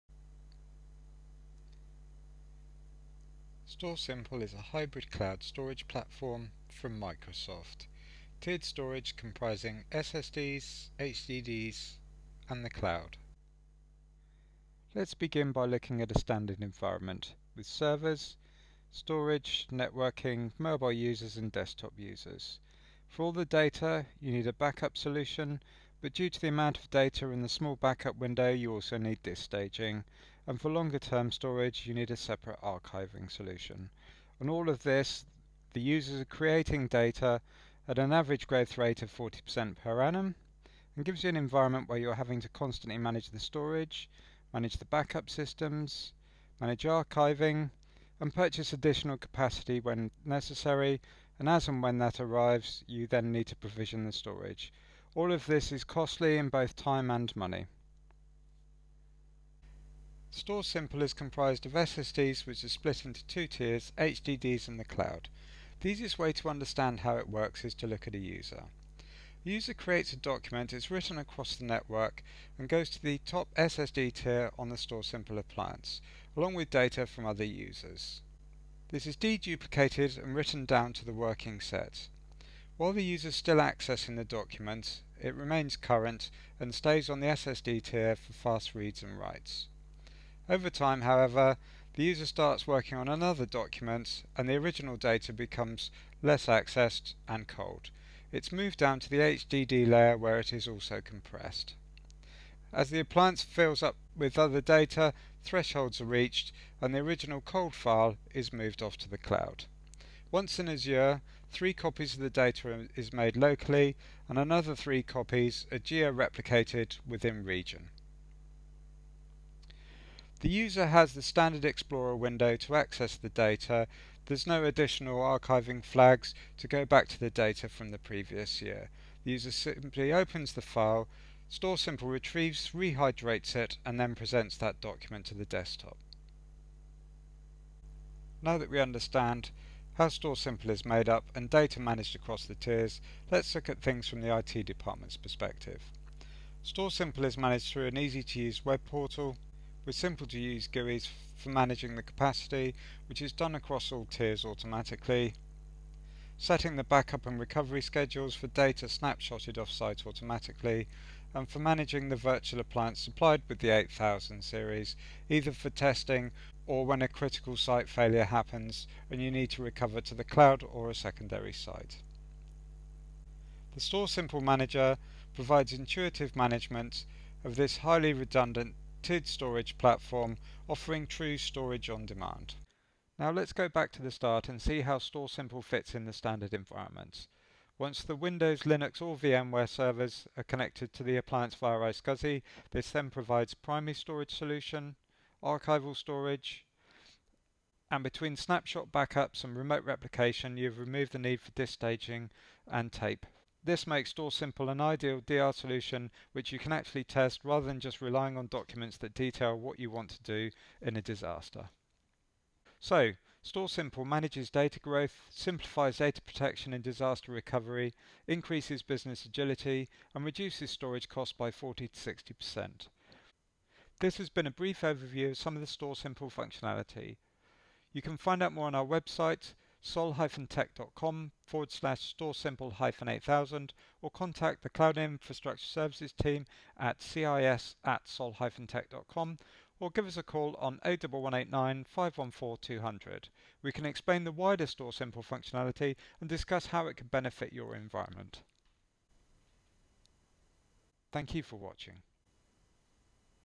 Microsoft Azure StorSimple Introduction: How it works
Microsoft Azure StorSimple 8000 series, an introduction to Microsoft's hybrid cloud storage platform.
This video takes you through what StorSimple is, how it works and where it fits within an IT environment.

Sol-Tec is a Microsoft Azure StorSimple 8000 Partner